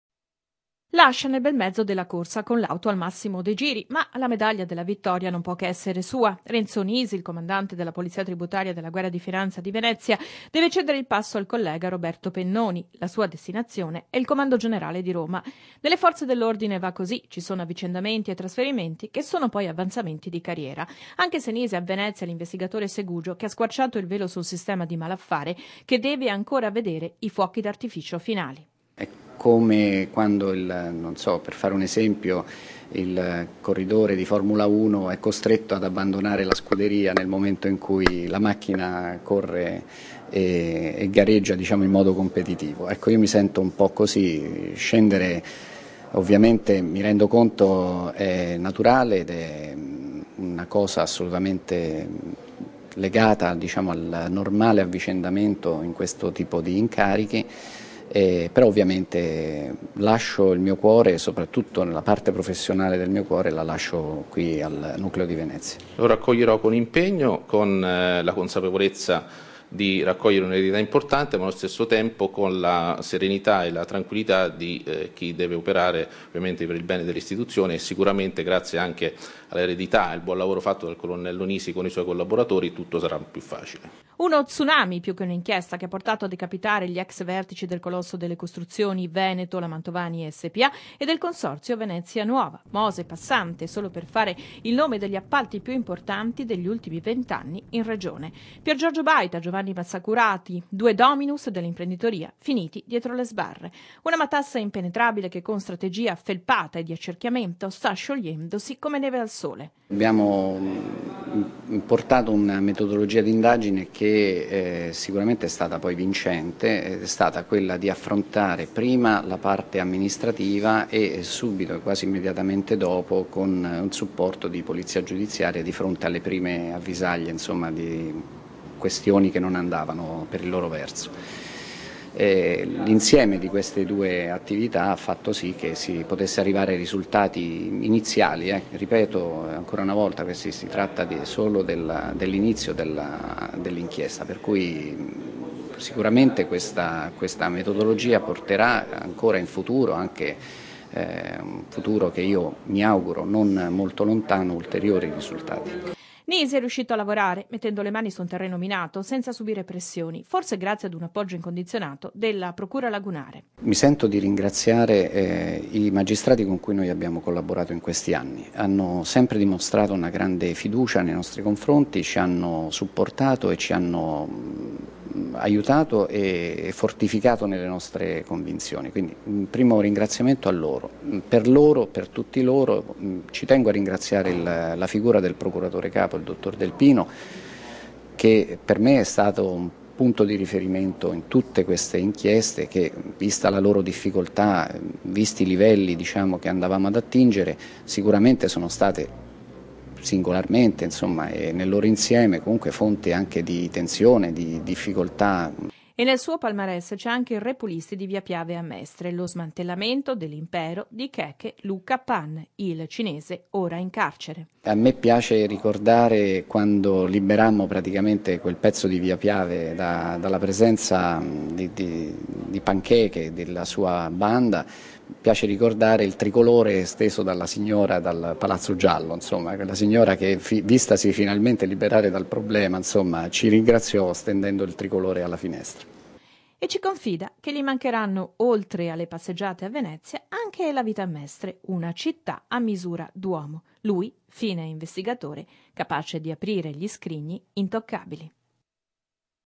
L'INDAGINE RESTA
VENEZIA: Promosso e trasferito il comandante della Polizia Tributaria di Venezia